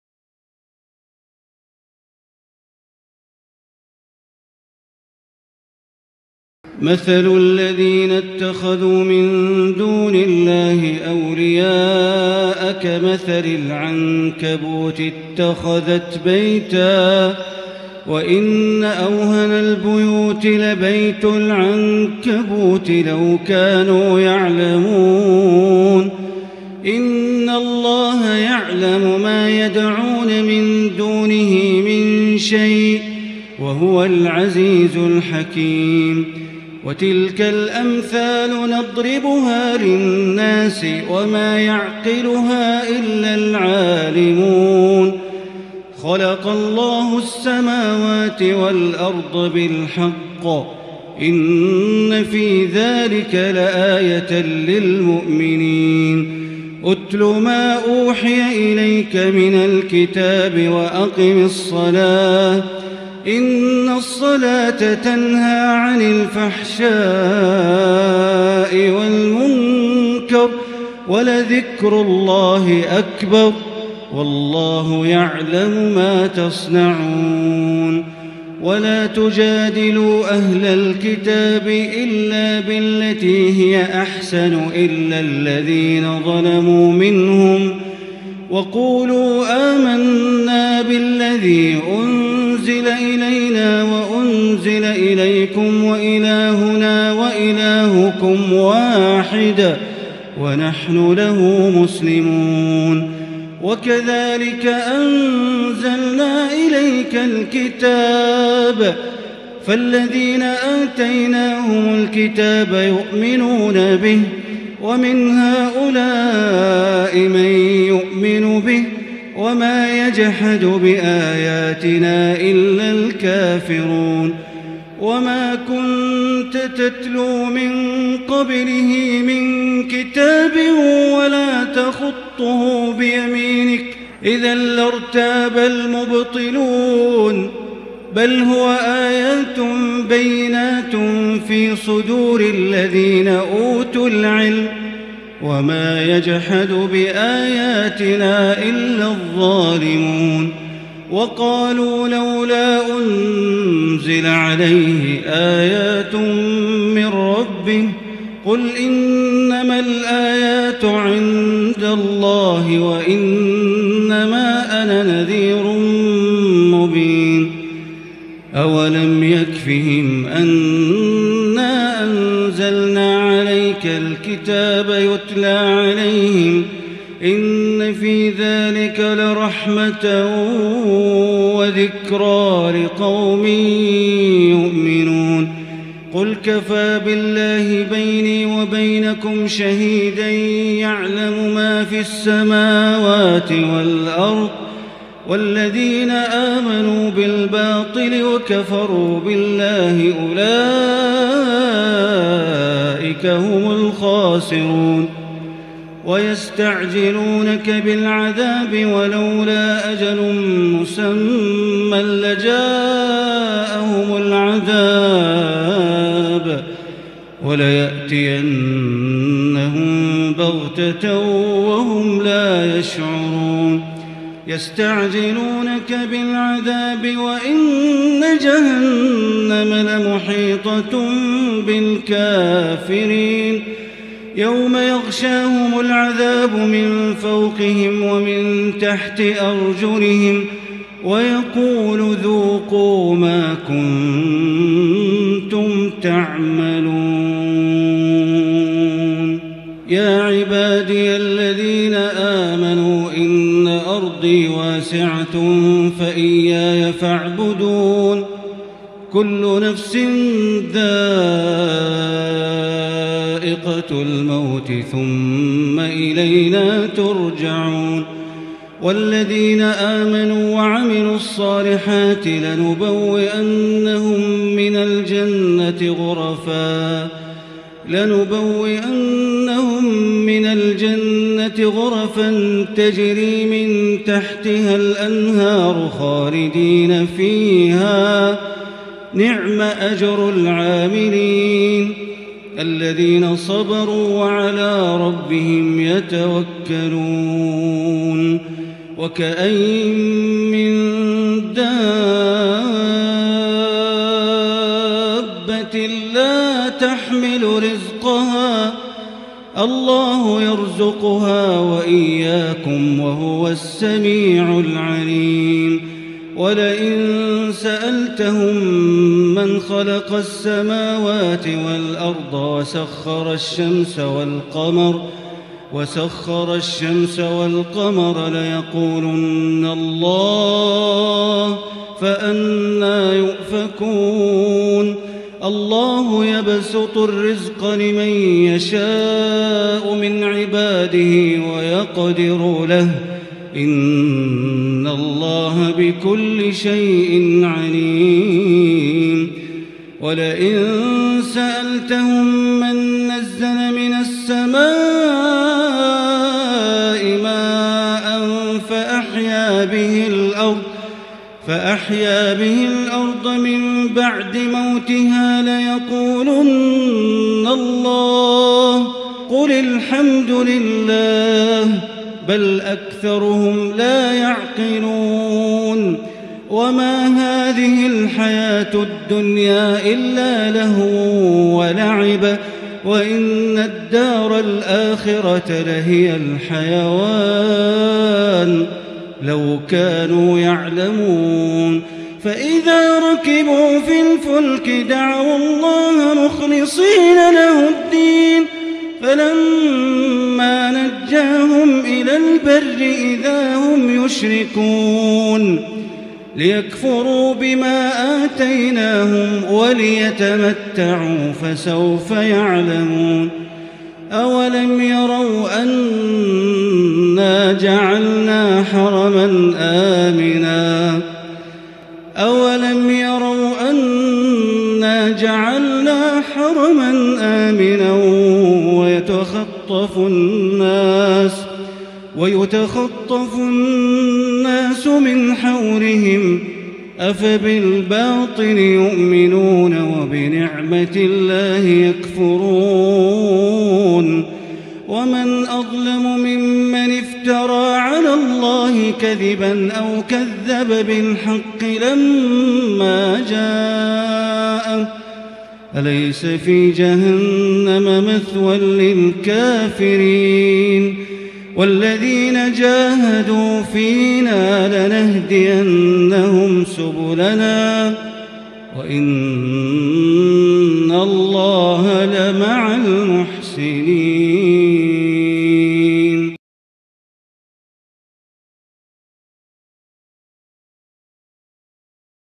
Makkah Taraweeh- 24th Night-Surah Al Ankabut (41-69)Sheikh Baleela with Arabic & English Translation
24th Night Of Makkah Taraweeh lead by the Sheikh Bandar Baleela-Surah Al Ankabut (41-69) with Arabic text and English Translationp